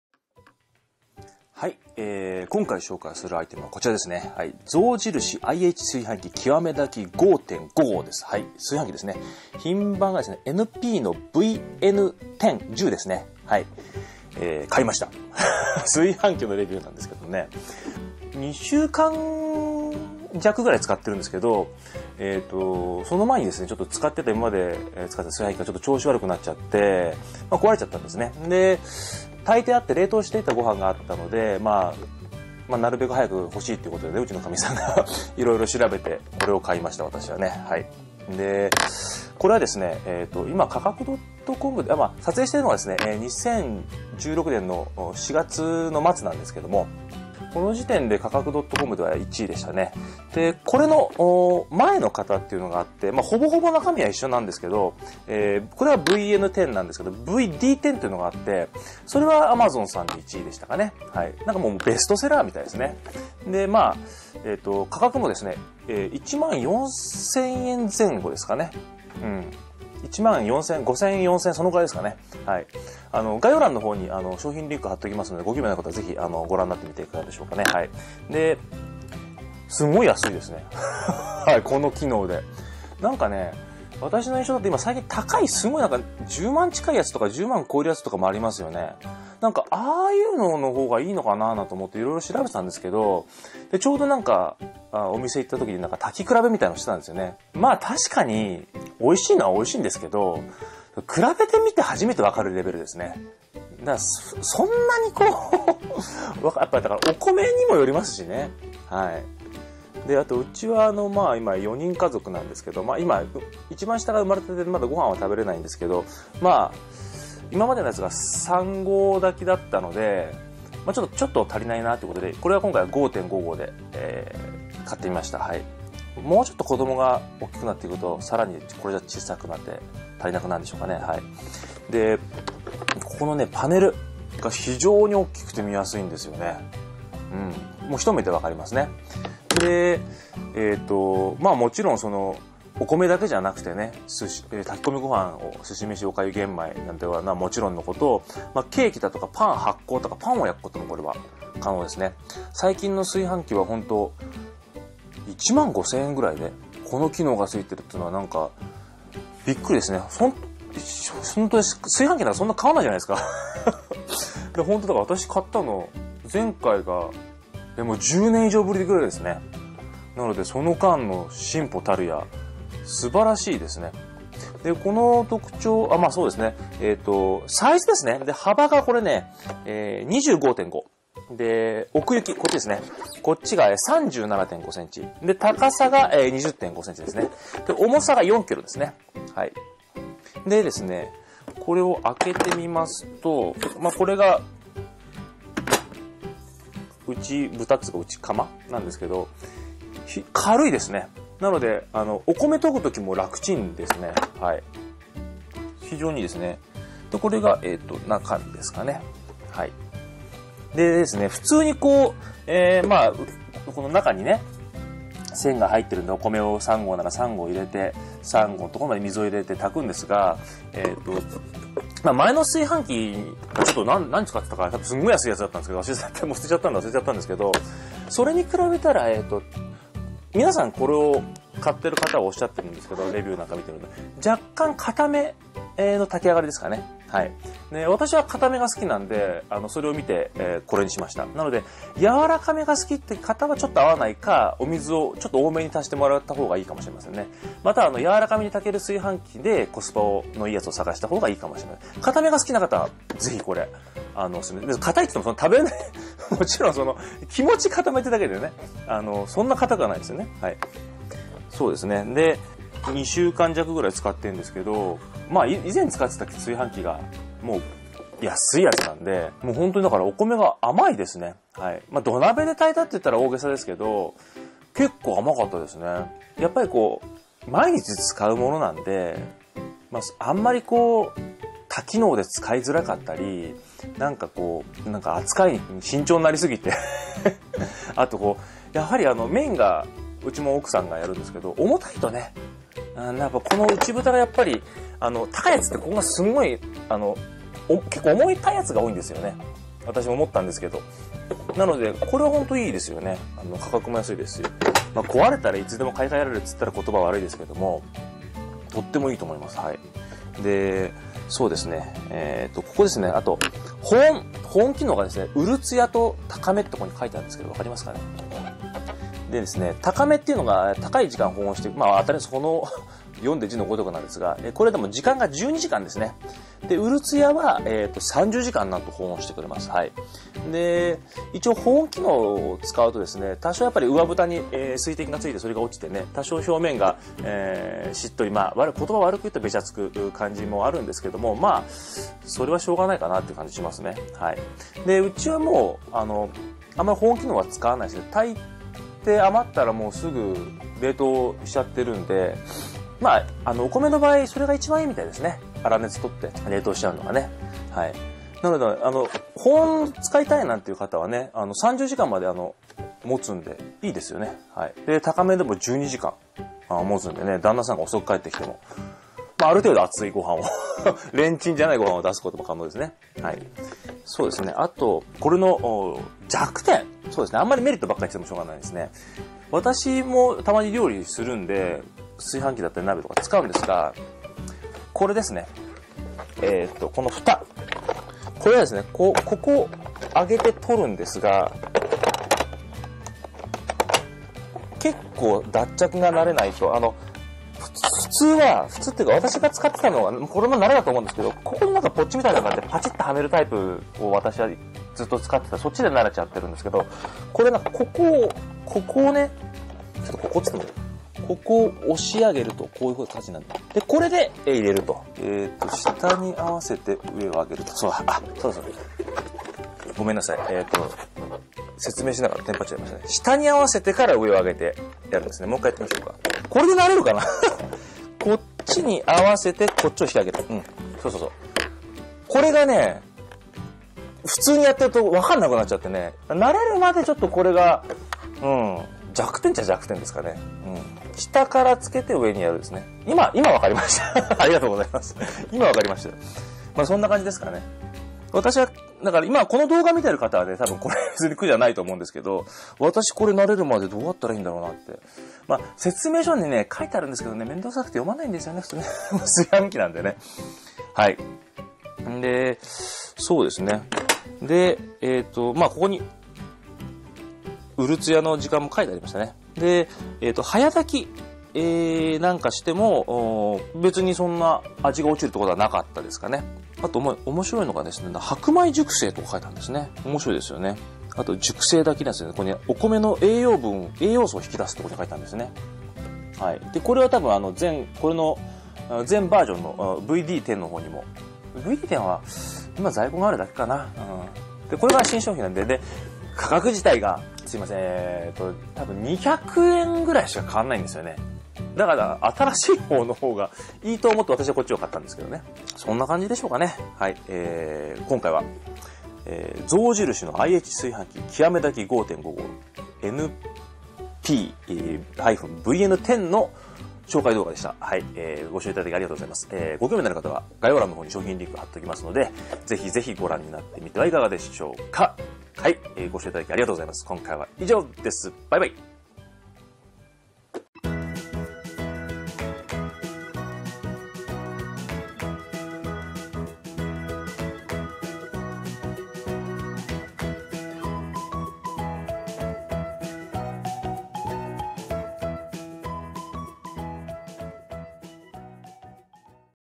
コスパ最高！象印 IH炊飯器 極め炊き 5.5合(NP-VN10)
炊飯器は迷うことなくコレ！固めの炊き上がりが好きな方はコレ一択！！

【アイテムリンク】